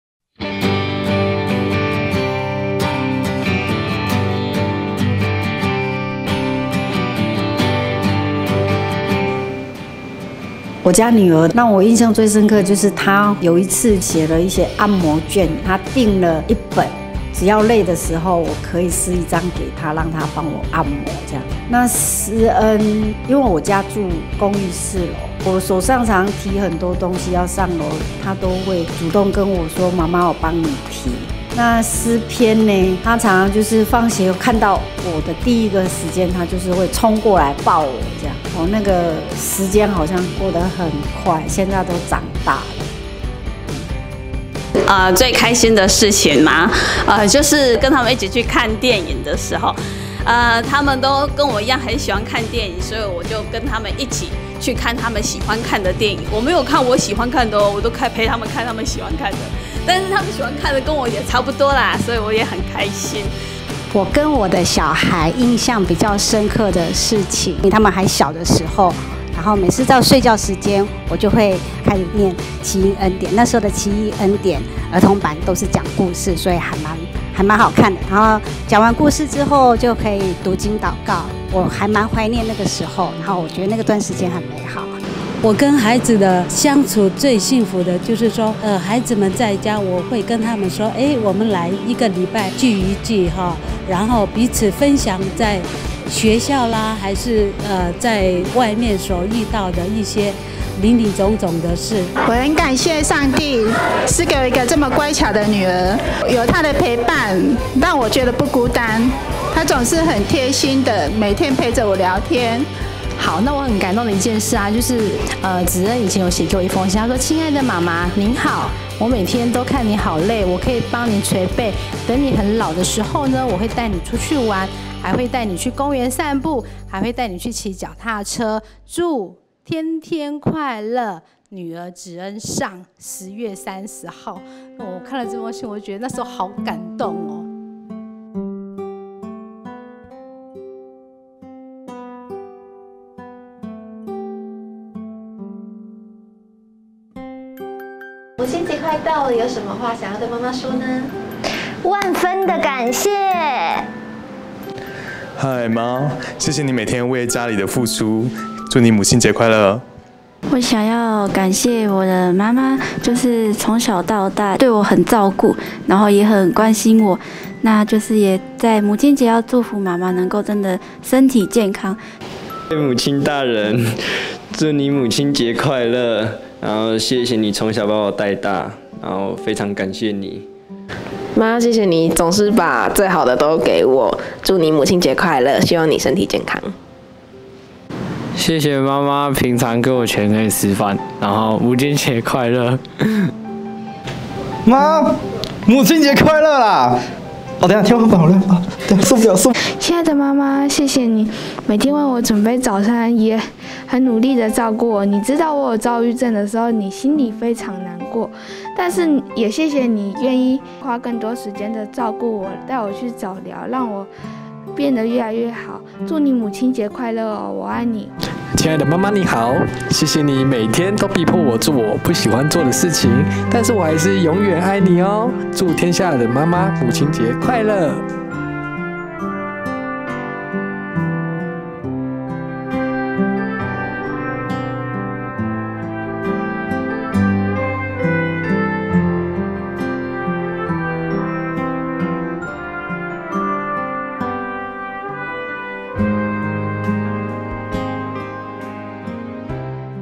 溝通GO不GO｜家，有您真好！【母親節特輯】
🙌 宇宙光『有愛無癮』全新企劃！
溝通GO不GO​ 系列，家，有您真好！【母親節特輯】

家，有您真好！
愛，因為有您無私的付出更完整。

👉一年一度的母親節，是獻上感恩的日子，
您有多久沒給媽媽一個擁抱了呢？

宇宙光在這美好的日子中，邀請影片中的每一位夥伴，
一同向辛苦的媽媽獻上感謝，
也邀請媽媽們分享兒女貼心的小故事。

祝福每一位媽媽母親節快樂，
願神的愛和恩惠臨到每個蒙福的家庭🧡